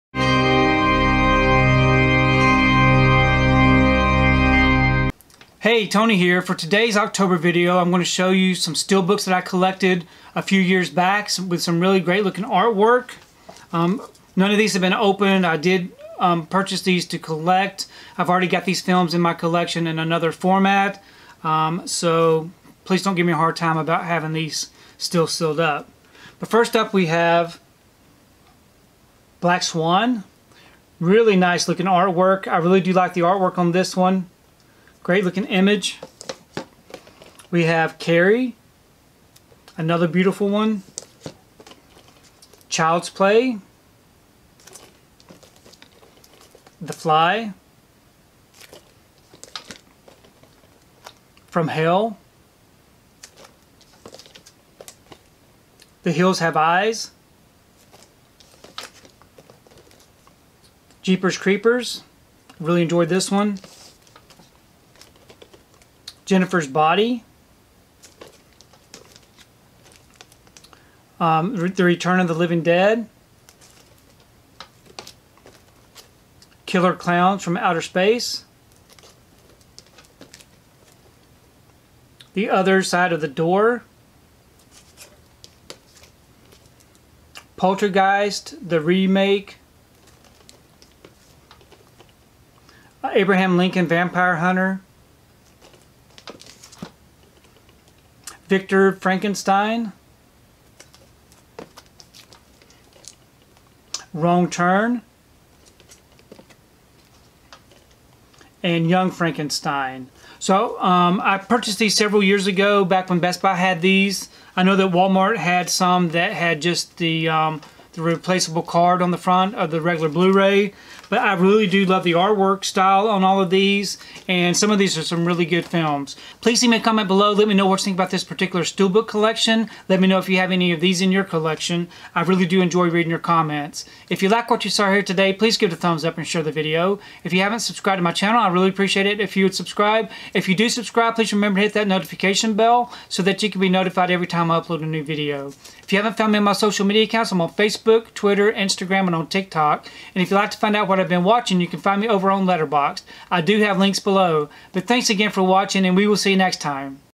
31 Days of Horror REWIND | Day 28 - My Orlando Arocena "Mexifunk" Horror Steelbook Collection
This is a video that I uploaded during the 31 Days of Horror back in 2021:  Check out these beautiful MGM/Fox Blu-ray steelbooks from celebrated artist Orlando Arocena that were released in October 2018.